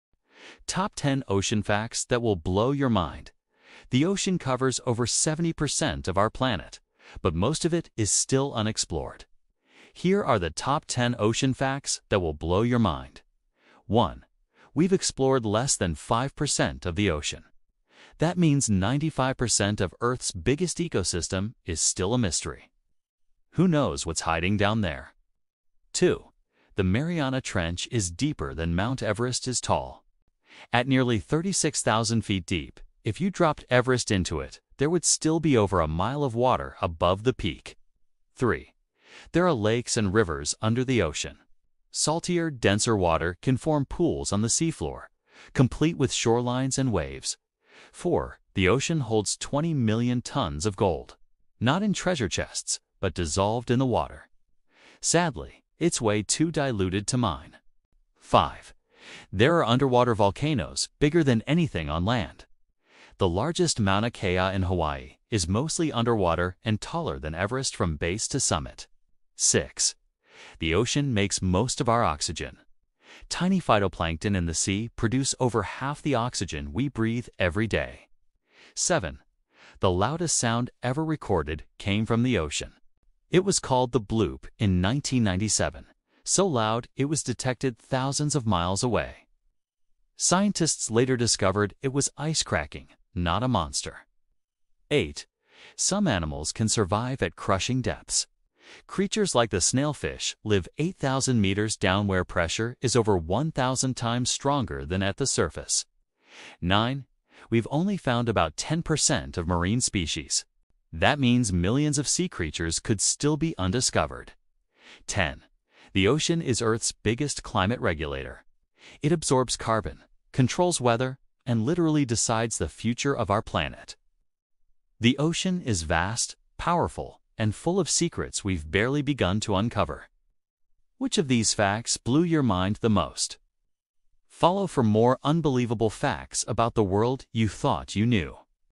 The Deep Sea Is Scarier Than You Think
From underwater lakes to gold dissolved in seawater, here are the Top 10 ocean facts that prove the sea is stranger than fiction. 🌊🐠
Which one surprised you most?